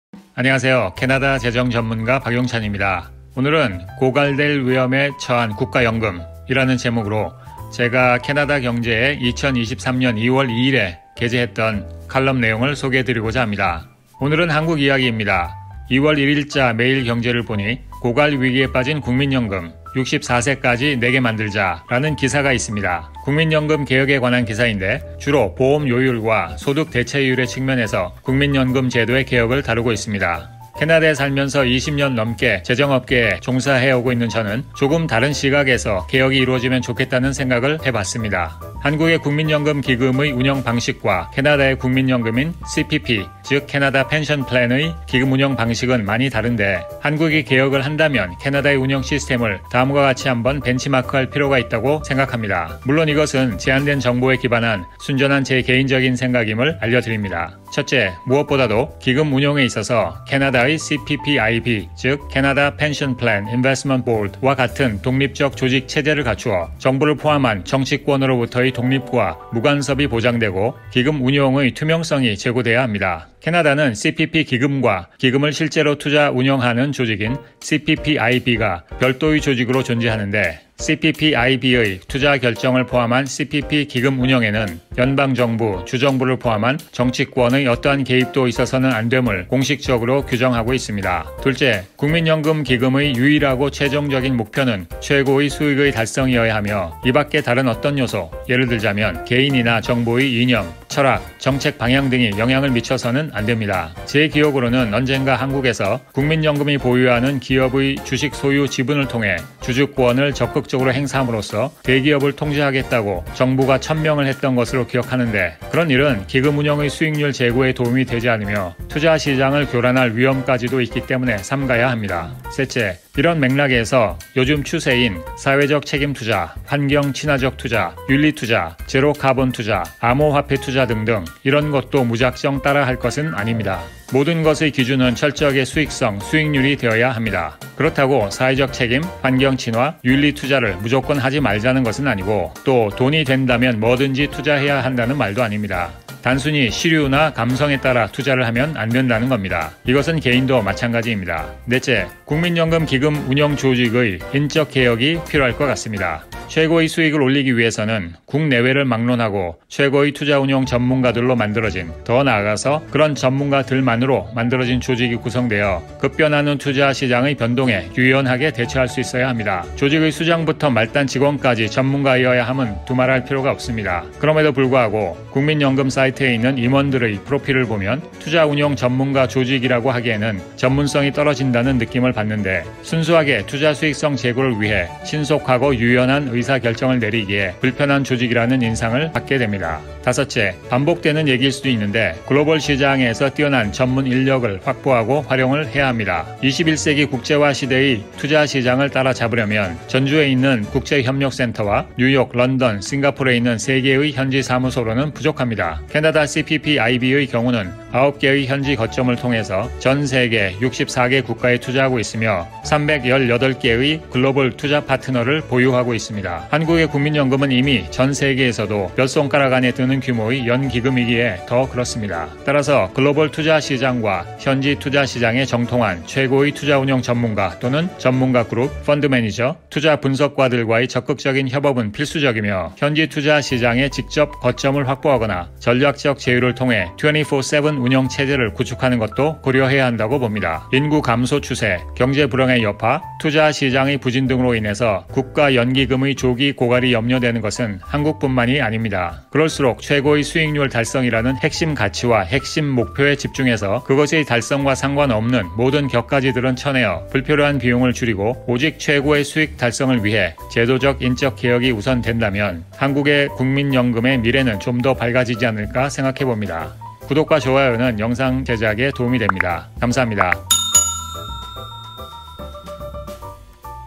연금 한국 국민연금의 본질적 문제는 수익률!
연기금 운영의 핵심 가치와 목표는 높은 수익률을 달성하여 은퇴한 국민들에게 부족함이 없이 연금을 지급하는 것입니다. 이를 위해 무엇보다도 한국 국민연금은 제도적인 개혁이 시급하다고 생각됩니다.

캐나다의 CPP (Canada Pension Plan)는 독립성이 보장된 전문가 조직인 CPPIB가 기금을 운영하는데, 한국도 제도적인 차원에서 CPPIB의 기금 운영 시스템을 벤치마크해서 현재 조직을 국제적이고 전문성과 신속성을 갖춘 전문가 조직으로 개혁해야 합니다. (블로그

(2023년 2월에 쓴 글이어서 3월에 발간된 CPPIB의 2023년 Annual Report 내용과 살짝 차이가 있는 부분이 있슴을 양해바랍니다.)